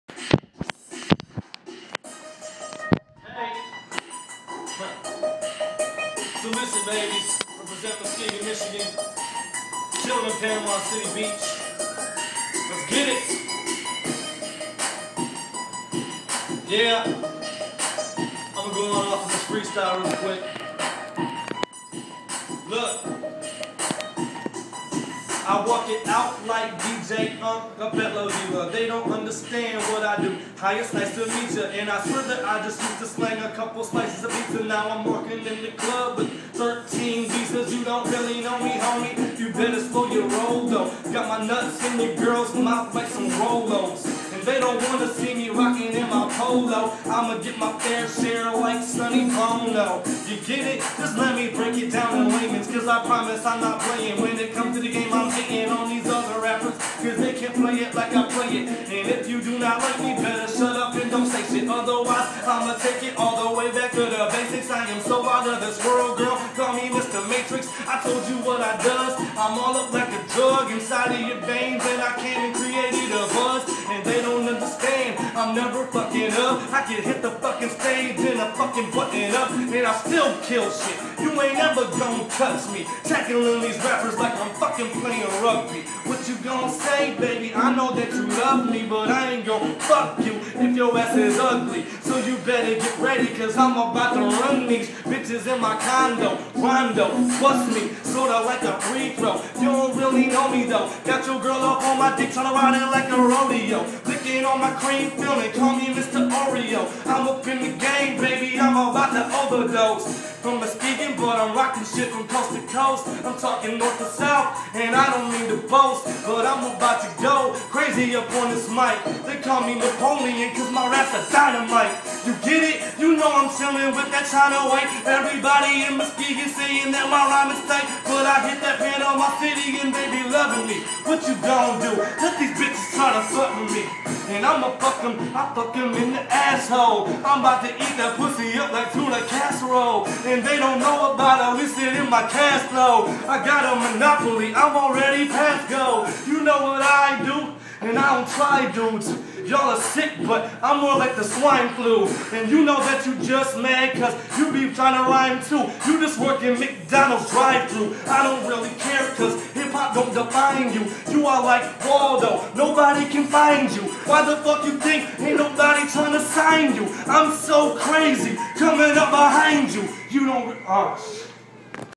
Illicit, freestyle in panama city beach
Illicit in his penthouse suite in panama city beach, straight off the dome